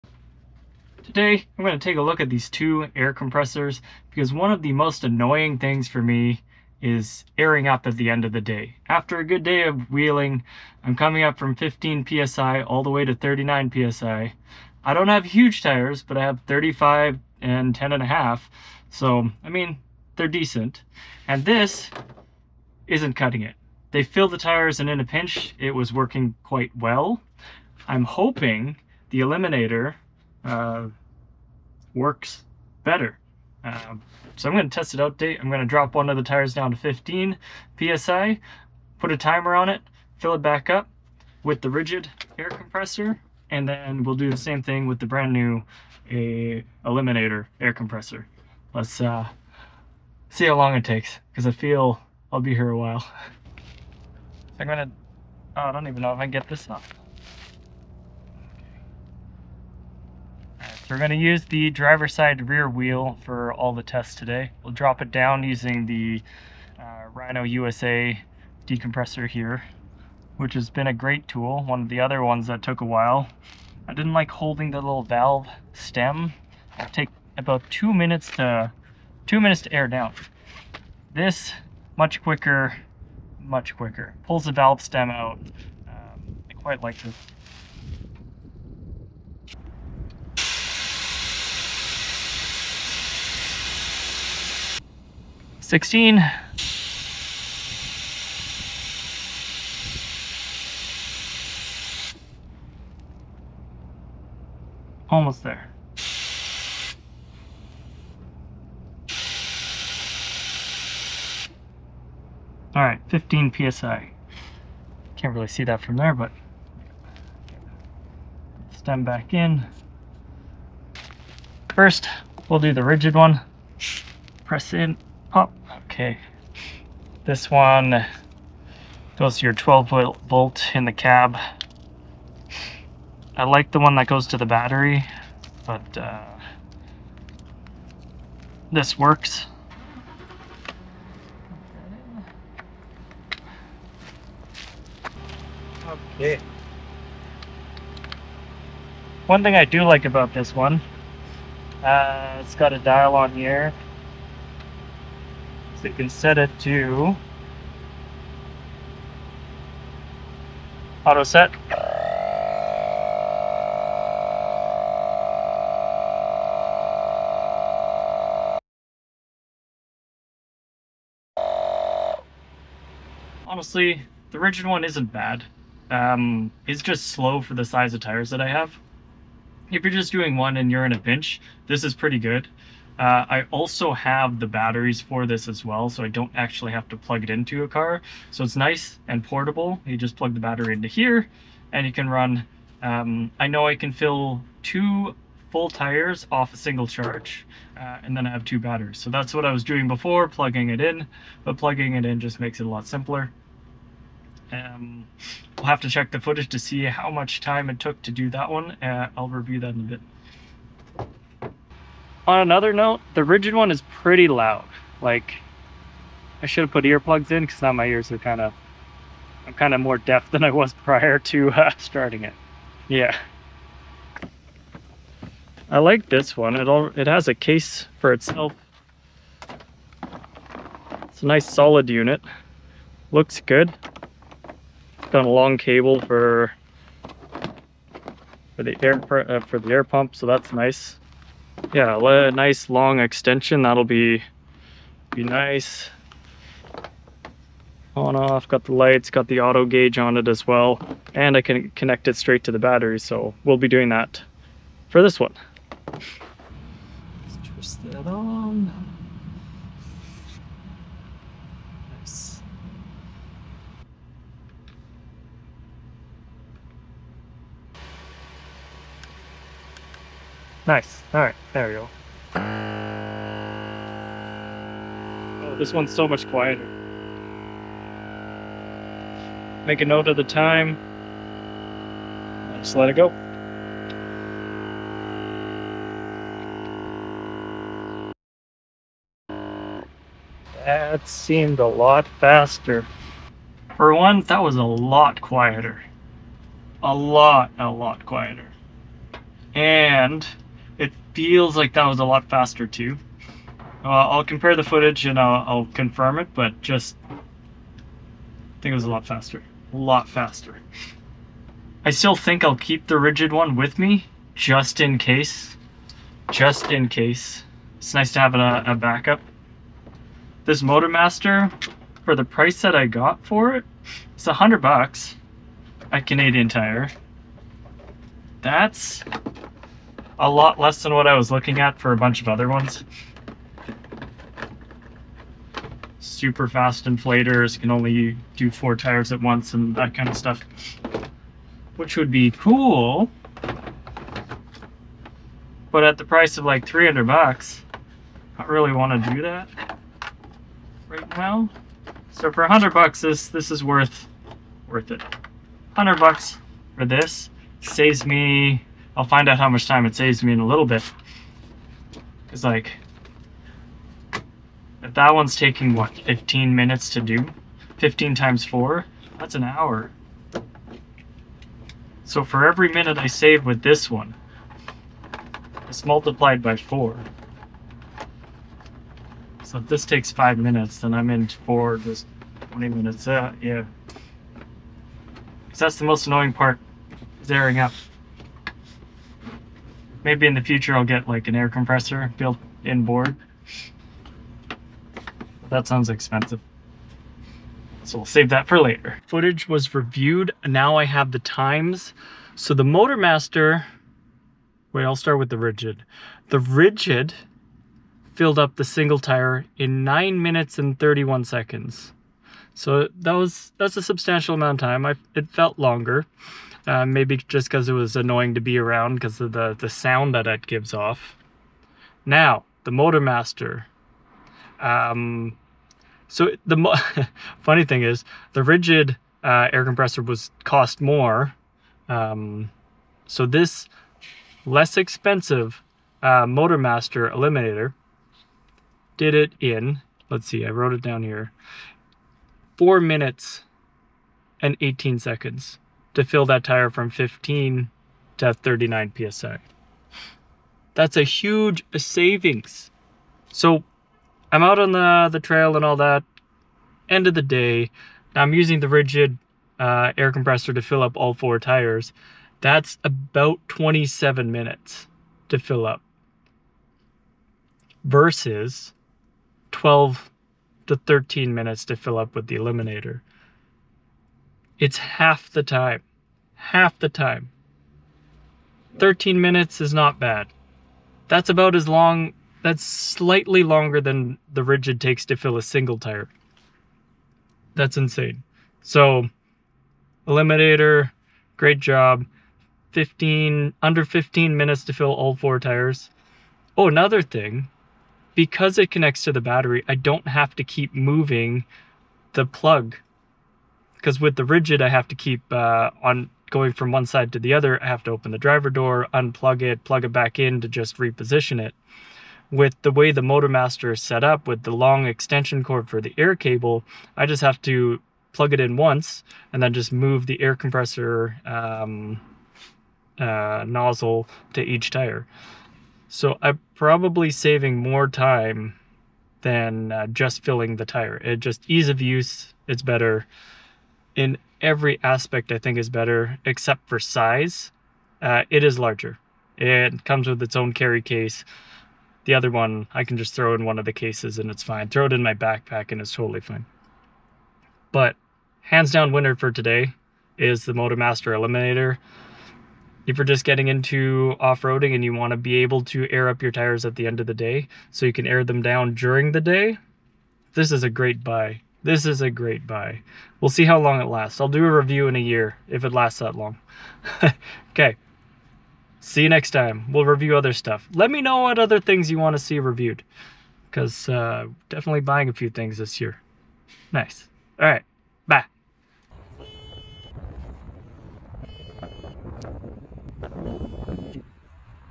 Motomaster VS Ridgid Tire inflators - Which one is faster?
Testing and reviewing the Motomaster 12 V Heavy-Duty Portable Air Compressor / Tire Inflator against the Ridgid 18V Cordless High-Pressure Inflator with Digital Gauge.

Trying to save time reinflating my tires, I hope to see a significant time saving between the two Inflators.

The Ridgid Inflator is now available at $92 as of making this video. Last year when I picked up the Ridgid tool it was $139. So technically the Ridgid tool is  slightly cheaper by $16, but the time spent airing up is more than double. So spend the extra few bucks to save yourself a lot of time airing up.